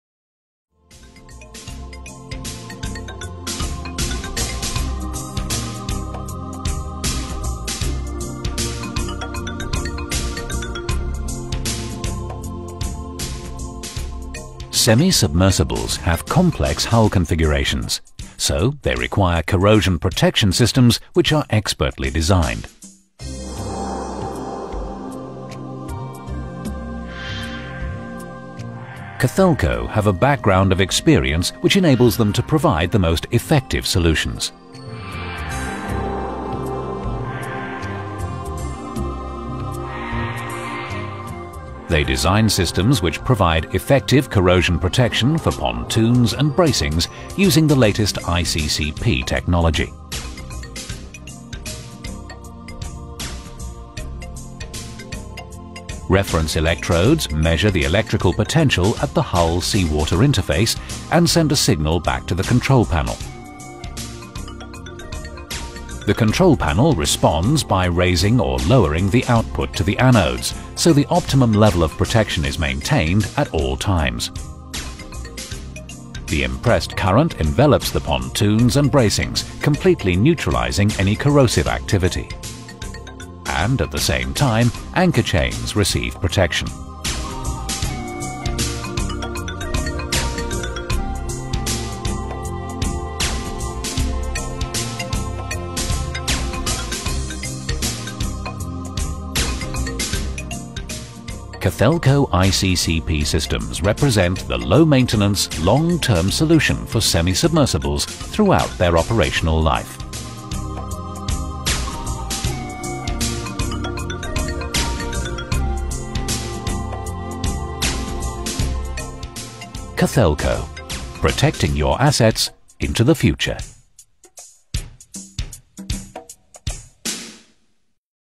Cathodic protection systems for Semi Submersibles
كيف تقاوم منصات الحفر البحرية العائمة الصدأ وهي في وسط مياه البحار والمحيطات؟؟
نظام متكامل من (الحماية الكاثودية) تتألف من الأنودات والأقطاب المرجعية السلكية الى لوحات التحكم. مصمم من
قبل المهندسين ذوي الخبرة ...التفاصيل في الفيديو التوضيحي التالي.
By : cathelco12